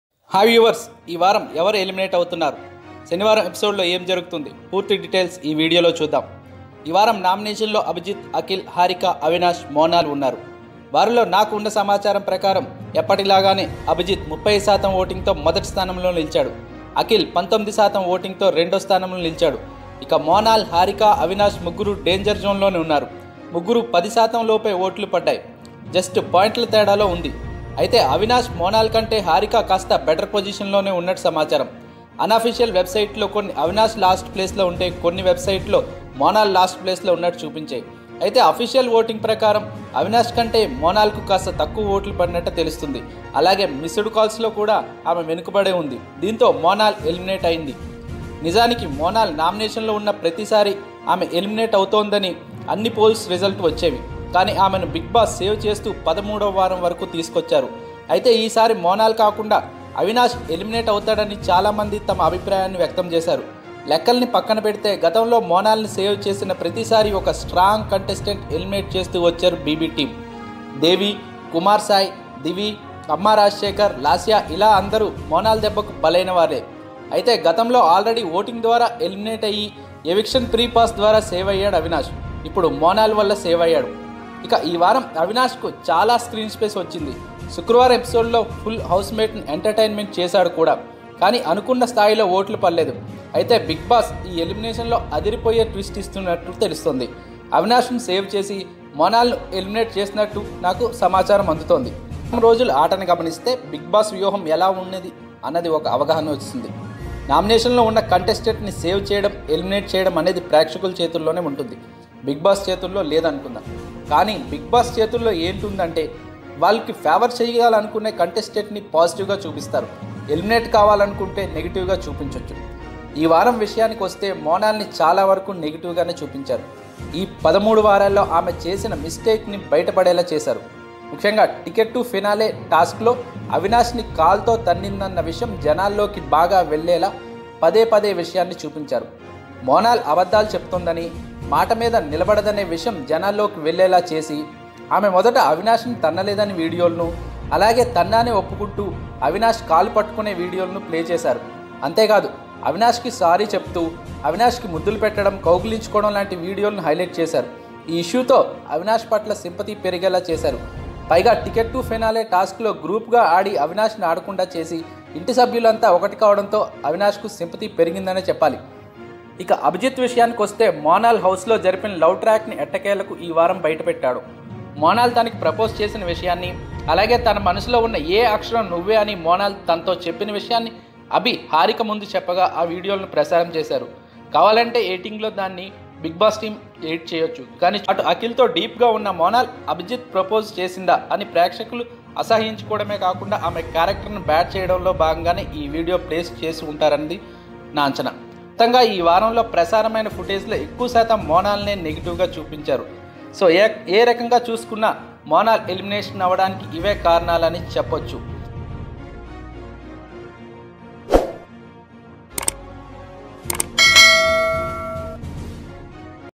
Monal Elimination confirmed || ఎట్టకేలకు బటయకు పంపిన బిగ్ బాస్ || కారణాలు ఏంటి?
నిజానికి మోనాల్ నామినేషన్స్‌లో ఉన్న ప్రతిసారి.. ఆమే ఎలిమినేట్ అవుతుందని అన్ని పోల్స్‌లో రిజల్ట్ వచ్చేది. కానీ ఆమెను బిగ్ బాస్ సేవ్ చేస్తూ 13వ వారం వరకూ తీసుకువచ్చారు. అయితే ఈసారి మోనాల్ కాకుండా అవినాష్ ఎలిమినేట్ అవుతాడని చాలా మంది తమ అభిప్రాయాన్ని వ్యక్తపరిచారు. లెక్కల్ని పక్కనపెడితే.. గతంలో మోనాల్‌ని సేవ్ చేసిన ప్రతిసారి ఒక స్ట్రాంగ్ కంటెస్టెంట్ ఎలిమినేట్ చేస్తూ వచ్చారు. దేవి, కుమార్ సాయి, దివి, అమ్మా రాజశేఖర్, లాస్య ఇలా అందరూ మోనాల్ దెబ్బకు బలైనవాళ్లే. అయితే గతవారం ఆల్రెడీ ఓటింగ్ ద్వారా ఎలిమినేట్ అయ్యి ఎవిక్షన్ ఫ్రీ పాస్ ద్వారా సేవ్ అయ్యాడు అవినాష్.

ఇక ఈవారం అవినాష్ కు చాలా స్క్రీన్ స్పేష్ వచ్చింది.. శుక్రవారం ఎపిసోడ్ లో  ఫుల్ హౌస్ మేట్స్ అందర్నీ ఎంటర్ టైన్ చేశాడు కూడా.. కానీ అనుకన్న స్థాయిలో ఓట్లు పడలేదు. అయితే బిగ్ బాస్ ఈ ఎలిమినేషన్‌లో అదిరిపోయే ట్విస్ట్ ఇవ్వబోతున్నట్టు తెలుస్తోంది. అవినాష్‌ని సేవ్ చేసి మోనాల్‌ని ఎలిమినేట్ చేసే సమాచారం.

గత వారం రోజులుగా ఆటను గమనిస్తే.. బిగ్ బాస్ వ్యూహం ఎలా ఉందన్నదానిపై ఒక అవగాహనకు రావొచ్చు. నామినేషన్స్‌లో ఉన్న కంటెస్టెంట్స్‌ని సేవ్ చేయడం ఎలిమినేట్ చేయడం అనేది ప్రేక్షకుల చేతుల్లోనే ఉంది.. బిగ్ బాస్ చేతుల్లో లేదని అనుకుందాం. కానీ బిగ్ బాస్ చేతుల్లో ఉన్నది ఏంటంటే.. వాళ్లు ఫేవర్ చేయాలనుకున్న కంటెస్టెంట్‌ని పాజిటివ్‌గా చూపించి.. ఎలిమినేట్ కావాలనుకుంటున్న కంటెస్టెంట్‌ని నెగిటివ్‌గా చూపిస్తుంటారు.

ఈవారం విషయానికి వస్తే.. మోనాల్‌ని చాలావరకూ నెగిటివ్‌గానే చూపించారు. ఈ 13 వారాల్లో ఆమె చేసిన మిస్టేక్స్‌ని బట్టబయలు అయ్యేలా చేశారు. ‘టికెట్ టు ఫినాలే’ టాస్క్‌‌లో అవినాష్‌ని కాలితో తన్నిందన్న విషయం జనంలోకి బాగా వెళ్లేలా పదే పదే ఆ విషయాన్నే చూపించారు. మోనాల్ అబద్ధాలు చెప్తుందని.. మాట మీద నిలబడదనే విషయం జనంలోకి వెళ్లేలా చేసి.. ఆమె మొదట అవినాష్‌ని తన్నలేదన్న వీడియోలను.. అలాగే తన్నానని ఒప్పుకుంటూ అవినాష్ కాళ్లు పట్టుకునే వీడియోలను ప్లే చేశారు. అంతేకాదు.. అవినాష్‌కి సారీ చెప్తూ మోనాల్.. అవినాష్‌కి ముద్దులు పెట్టడం.. కౌగిలించుకుకోవడం లాంటి వీడియోలను హైలైట్ చేశారు. ఈ ఇష్యూతో అవినాష్ పట్ల సింపథీ పెరిగేలా చేశారు. పైగా ‘టికెట్ టు ఫినాలే’ టాస్క్‌‌లో గ్రూప్‌గా ఆడి అవినాష్‌ను ఆడకుండా చేసి.. ఇంటి సభ్యులంతా ఒకటి కావడంతో అవినాష్‌కి సింపథీ పెరిగిందనే చెప్పాలి.

ఇక అభిజిత్ విషయానికి వస్తే.. మోనాల్ హౌస్‌లో జరిపిన లవ్ ట్రాక్‌ని ఎట్టకేలకు ఈ వారం బయటపెట్టాడు. మోనాల్ తనకి ప్రపోజ్ చేసిన విషయాన్ని.. అలాగే తన మనసులో ఉన్న A అక్షరం నువ్వే అని మోనాల్ తనతో చెప్పిన విషయాన్ని అభి హారిక ముందుకు చెప్పగా ఆ వీడియోలను ప్రసారం చేశారు. దీన్ని బట్టి అటు అఖిల్‌తో డీప్‌గా ఉన్న మోనాల్.. అభిజిత్‌ ప్రపోజ్ చేసిందా? అని ప్రేక్షకులు అసహ్యించుకోవడమే కాకుండా ఆమె క్యారెక్టర్‌ని బ్యాడ్ చేయడంలో భాగంగానే ఈ వీడియోను ప్లే చేసి ఉంటారనేది బలమైన కారణం. మొత్తంగా ఈవారంలో ప్రసారమైన ఫుటేజ్‌లో ఎక్కువ శాతం మోనాల్‌ని నెగిటివ్‌గానే చూపించారు.